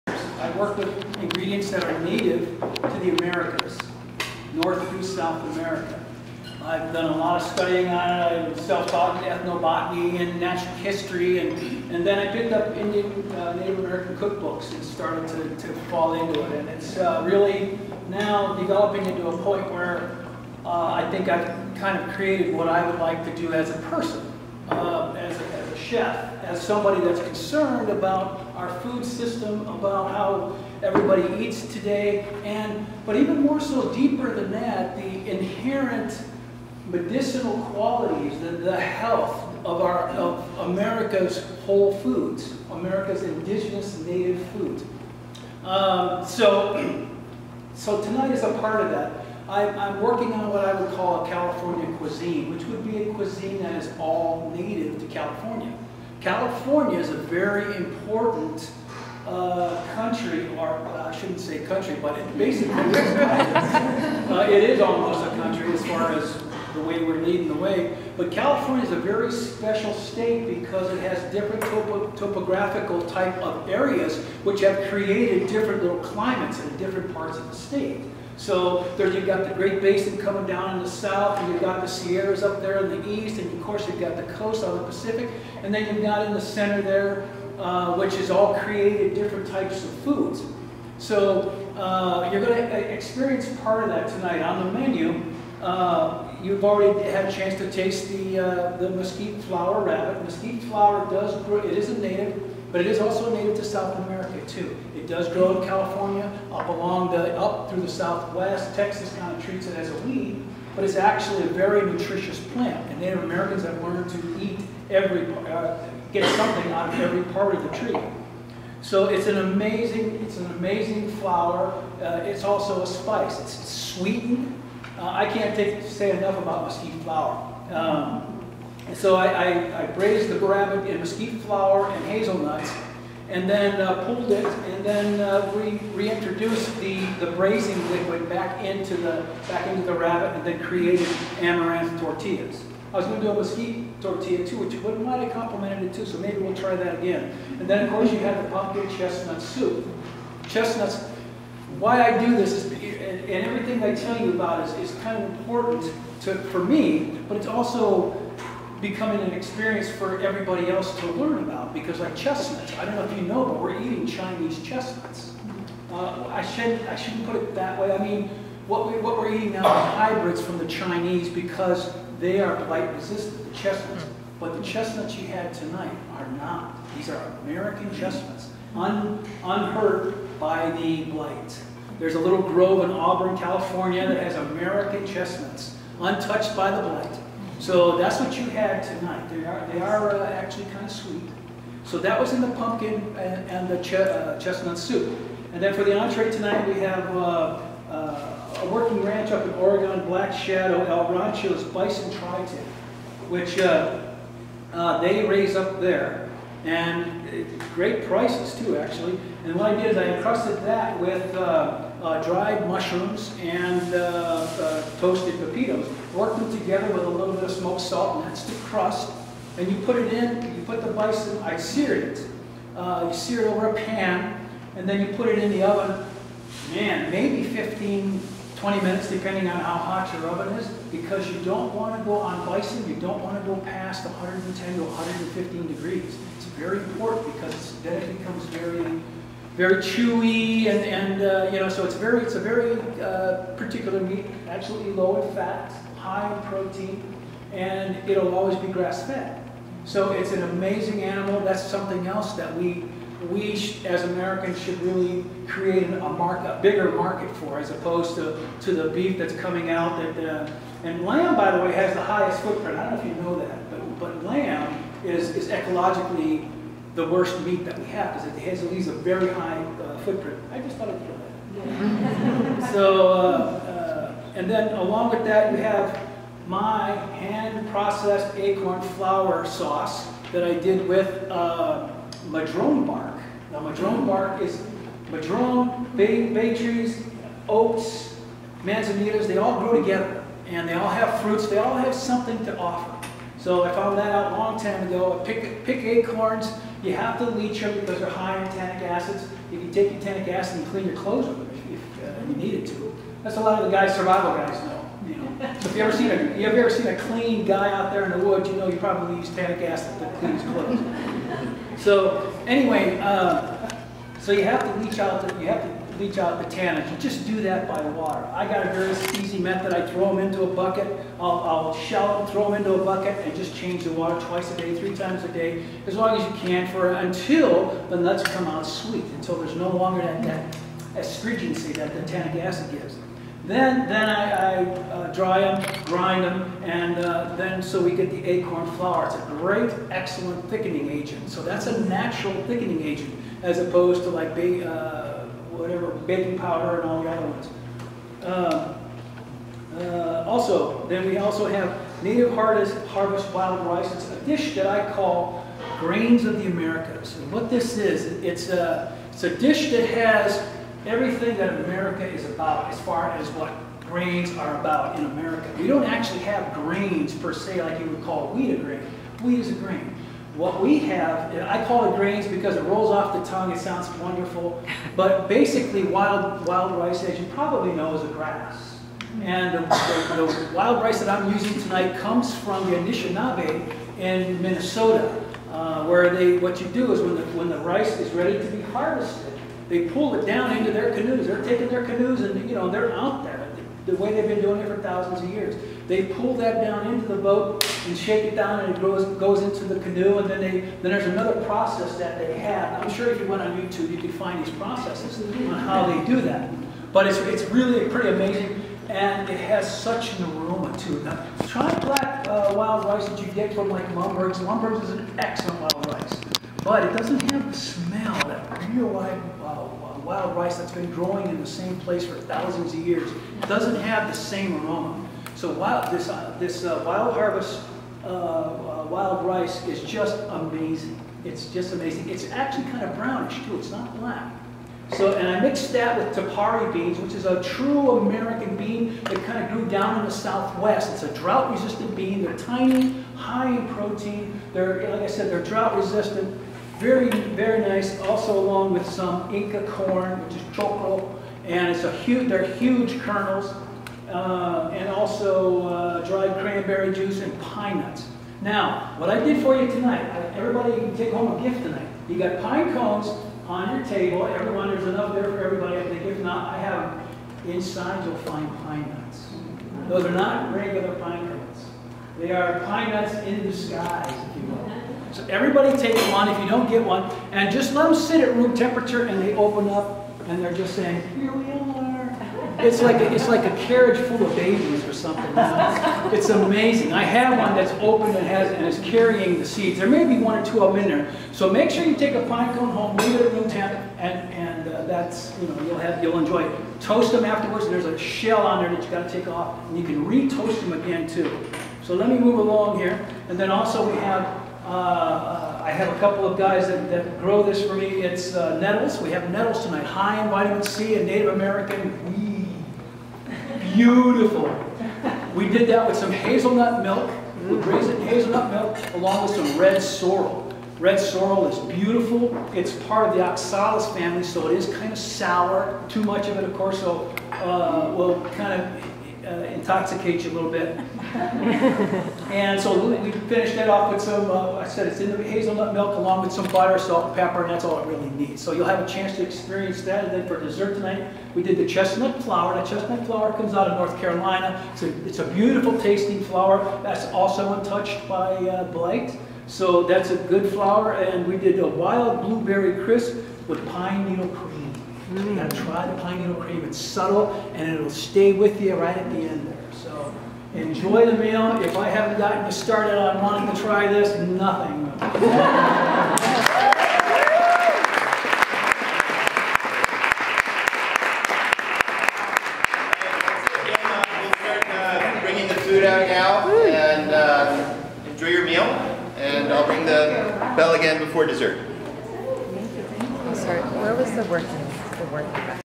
John Farais Describes His CA and The Americas' Harvest Menu with a Short Lecture (2015)
A Good People Dinner, October 28, 2016, in San Francisco. John Farais describes the native ingredients on his menu with anecdotes, and history with a little humor and lots of passion.

The Menu:

Appetizer
Seared Mesquite Flour and Hazelnut Pulled Rabbit on an Amaranth Tortilla With Wild Plum Chutney
(Native California)

 Entrée
Black Shadow El Rancho’s Bison Tri Tip Encrusted with Dried Wild Mushrooms and Toasted Pepitos
(Plains, Meso America)
With John’s Hand-Processed Acorn Flour/Madrone Sauce ,
(California)

Grains of The Americas-Native Harvest Wild Rice, Quinoa, Inca Corn, Tepary Beans, Dried Cranberries, Pine Nuts (Anishinaabe, Peru, Southwest, Northeast US)

Star Route & County Line, Nettles, Dandelion Greens, Red Sorrel
Braised in Hazel Nut Milk
(The Americas)

Dessert
Chestnut Flour , Wild Blueberry Crisp with Pine Needle Cream

Chef John Farais’ Mission: To help People rediscover our proud, ancient history of native edibles, while awakening them to the nutritional, practical and medicinal values of native and all whole foods.